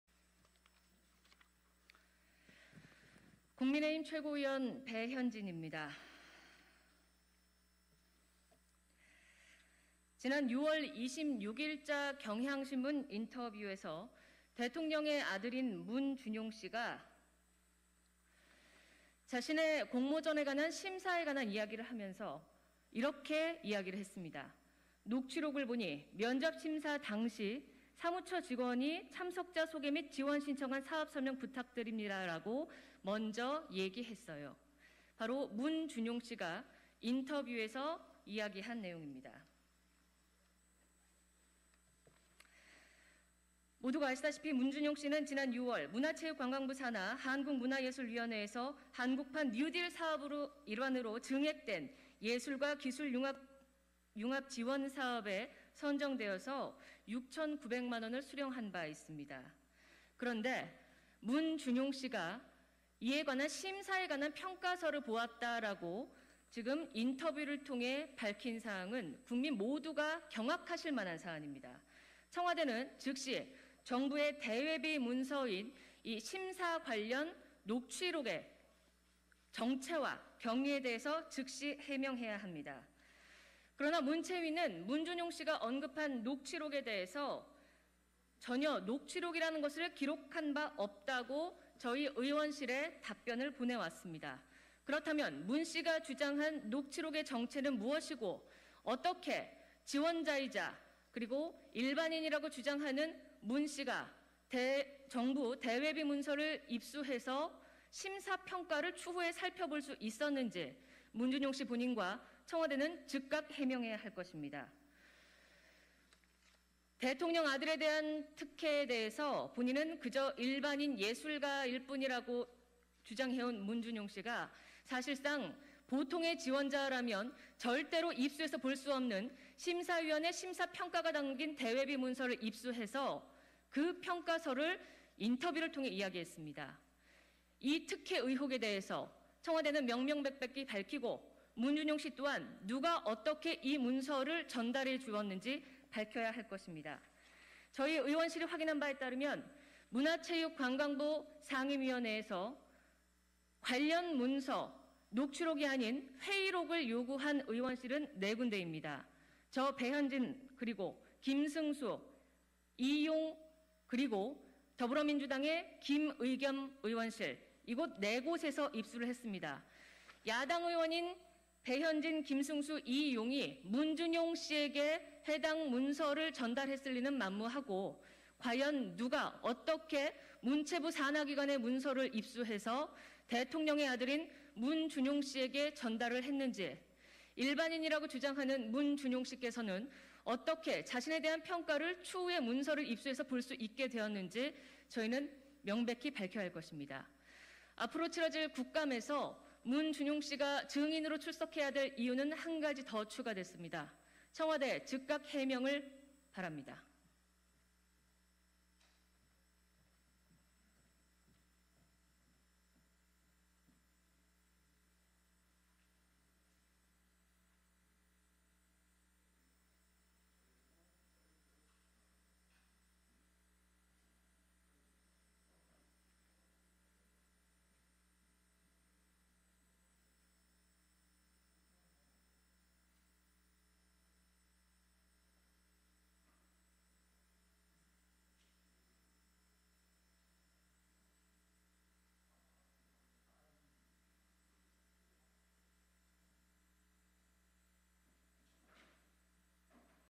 배현진 "문준용은 국가 대외비 심사위원 녹취록 입수 경위 이실직고 하든지, 국정감사에 나오든지!"
문준용 발언 논란, 국민의힘 배현진 최고위원이, 6월 29일 국회 소통관에서 기자회견을 열고 문재인 대통령 아들 준용 씨가 정부 기관인 한국문화예술위원회로부터 6천900만원의 지원금을 받는 심사 면접 당시 심사위원들의 녹취록을 입수한 것에 대해 대외비 문건을 입수한 경위를 밝히라며 강한 의혹을 제기했다.